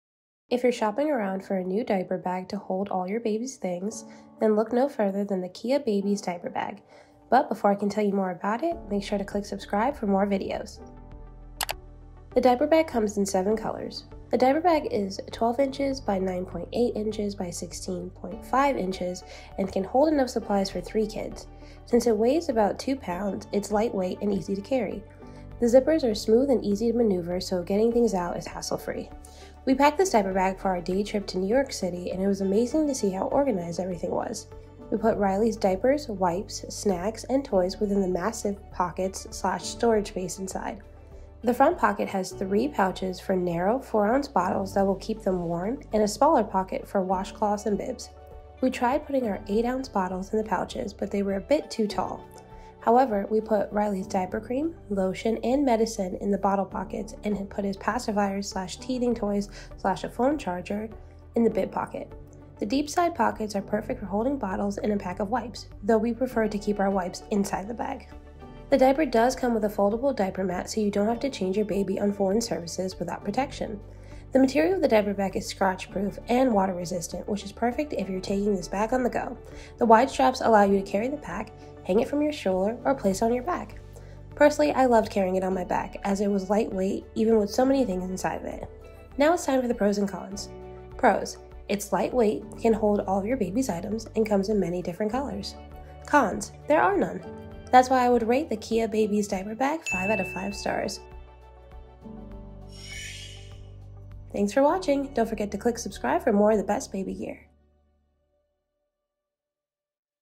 KeaBabies Diaper Bag
Today TTPM is reviewing the KeaBabies Diaper Bag. The perfect bag for all your needs . See more in this video review! For full review and shopping info

If you’re shopping around for a new diaper to hold your baby’s things, then look no further than the KeaBabies Diaper Bag.